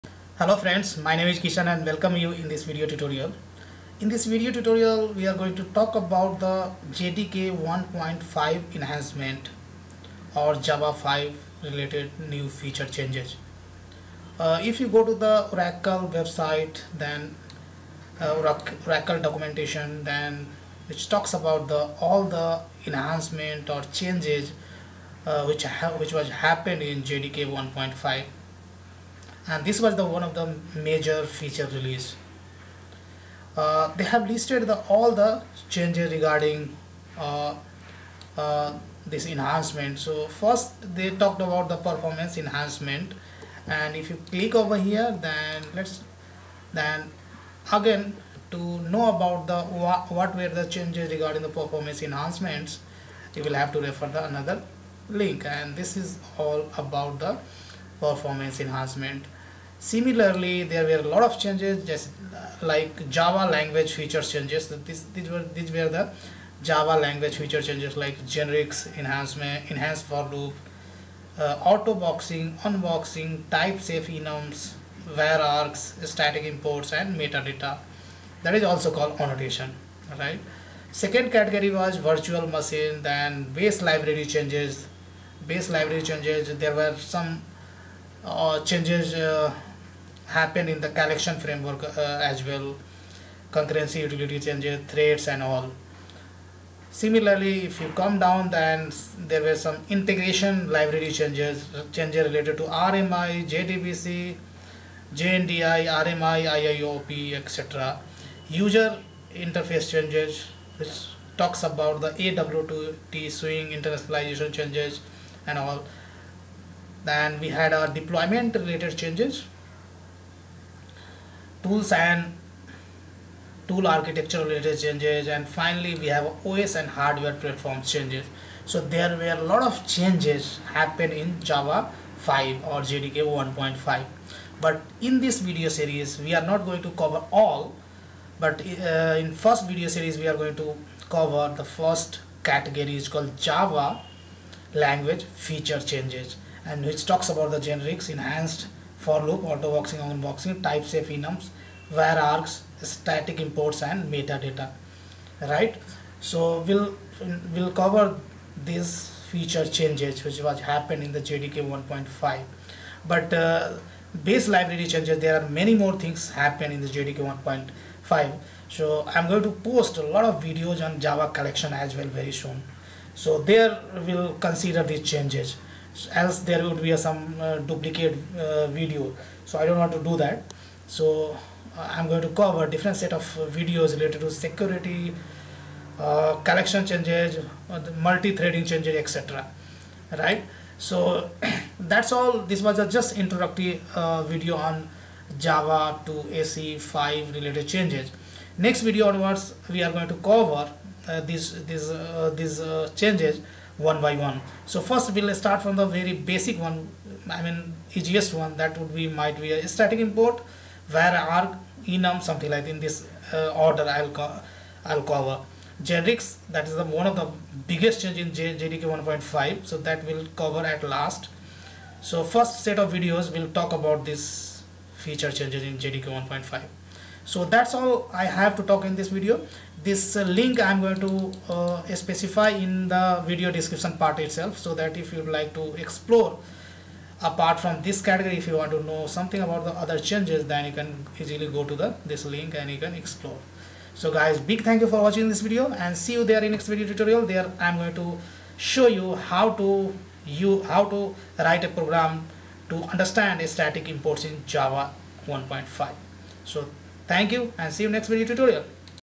Java 5 new features
In this video we will talk about java 5 new features.